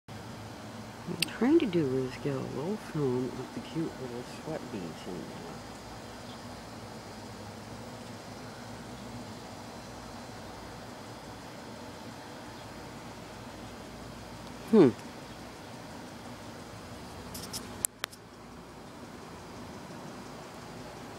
Cilantro and Sweat-Bees
You can hardly see the tiny sweat-bees at work...